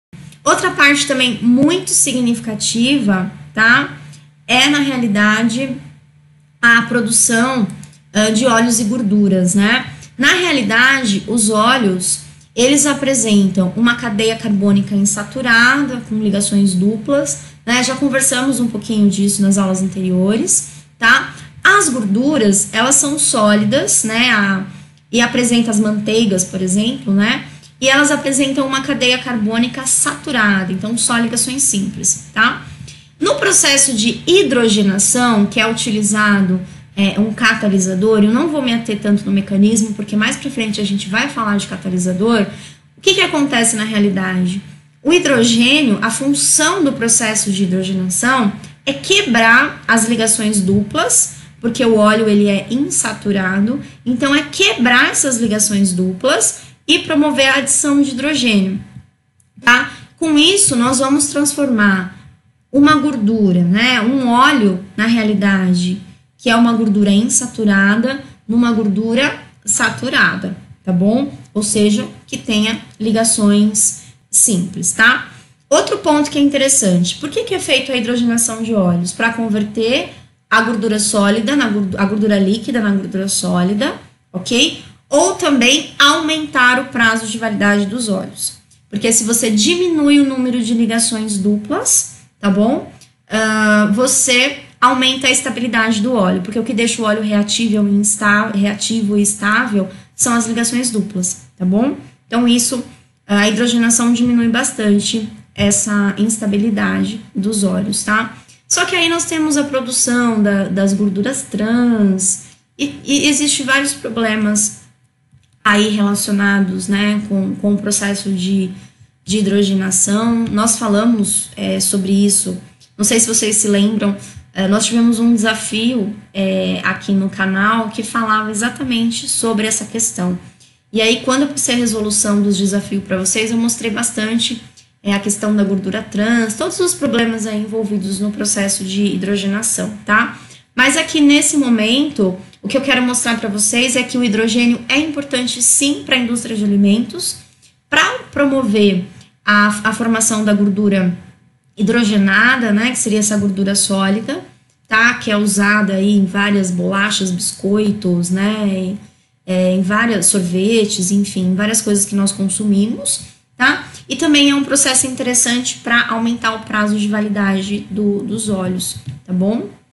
Uso de hidrogênio (H2) na hidrogenação de óleos
No vídeo de hoje, vamos explorar o interessante processo de hidrogenação de óleos vegetais. Vamos mergulhar nas etapas detalhadas desse processo químico, que envolve a adição de hidrogênio aos óleos vegetais insaturados para transformá-los em gorduras saturadas. Vamos discutir os catalisadores e as condições ideais de temperatura e pressão utilizados nessa reação. Além disso, vamos abordar as aplicações e importância da hidrogenação de óleos na indústria de alimentos, onde ela é usada para produzir margarinas, gorduras sólidas e produtos com maior estabilidade oxidativa. Prepare-se para desvendar os segredos do processo de hidrogenação de óleos e entender como ele contribui para a produção de alimentos mais saudáveis e duradouros.